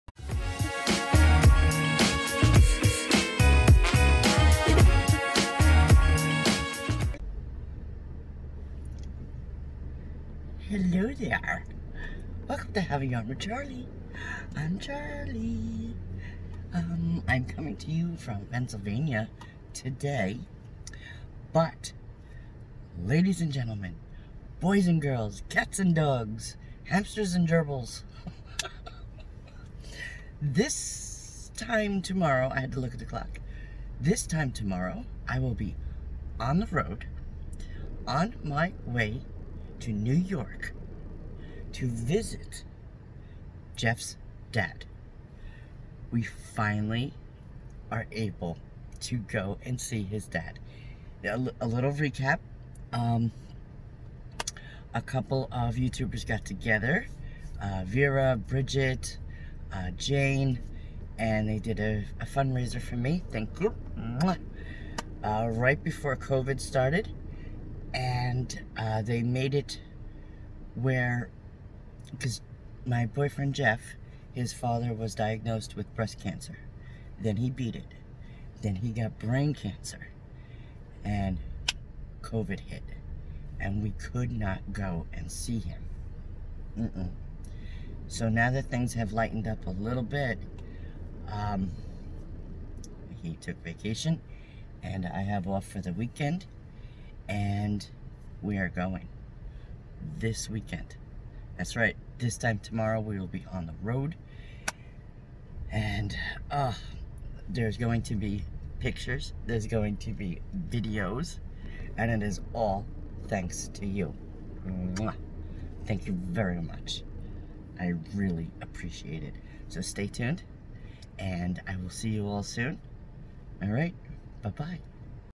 Going to see Jeff's DAD
41 Views - Oct 15, 2023
That's right, finally after waiting months and months because of covid-19 we are finally able to go and see Jeff's dad.
Jeff is so excited
We will definitely be taking pictures and videos